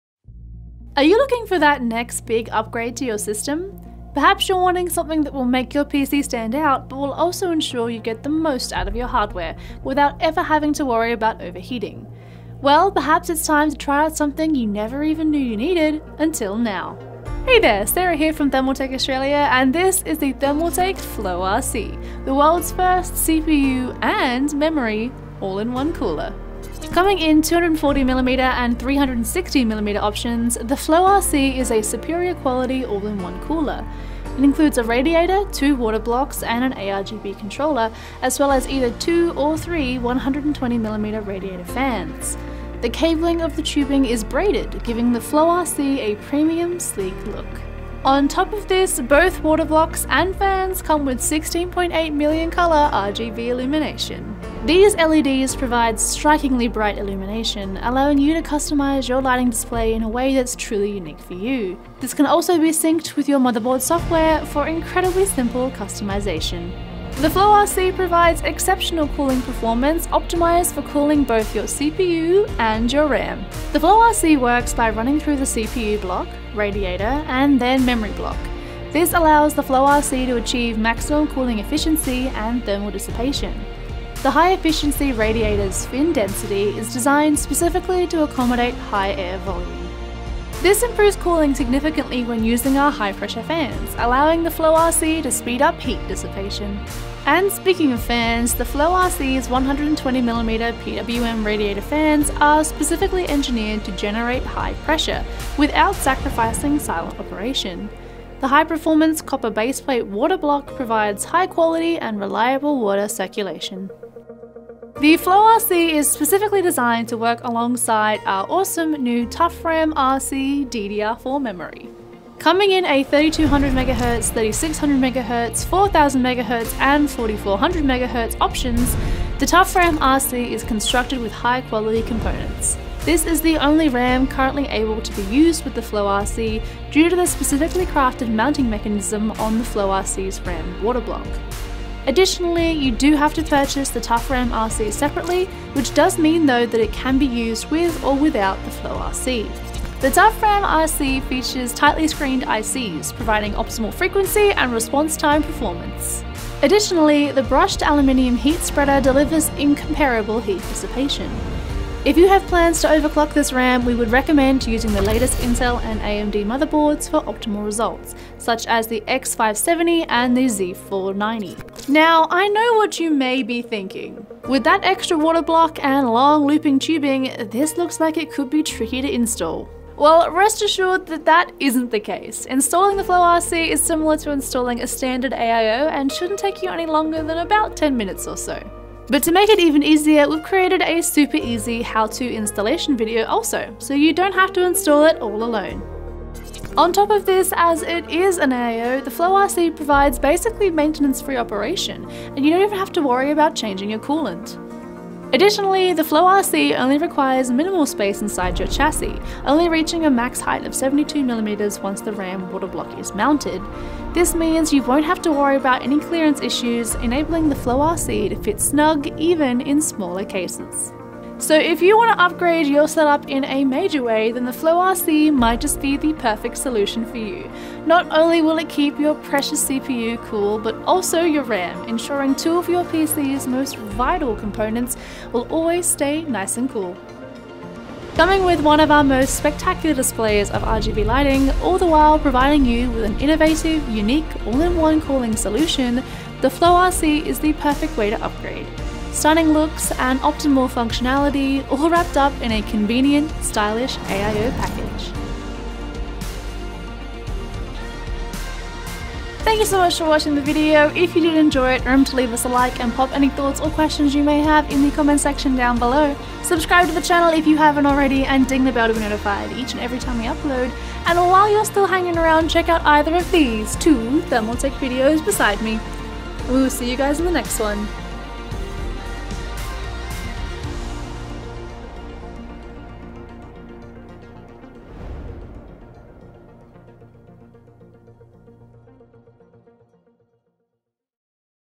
Thermaltake Floe RC CPU and RAM All In One LIQUID Cooler First Look
Floe RC is the world first-ever memory and liquid cooling all-in-one solution, providing exceptional cooling performance for both CPU and RAM; pairing exclusively with our TOUGHRAM RC memory modules. Built with high-quality components, the TOUGHRAM RC delivers high performance and superb heat dissipation with the fine brushed aluminum heatsink. Not only can the TOUGHRAM RC pair with the Floe RC CPU & Memory AIO liquid coolers, they can also be used individually.

-Buy on US Amazon
Floe RC Series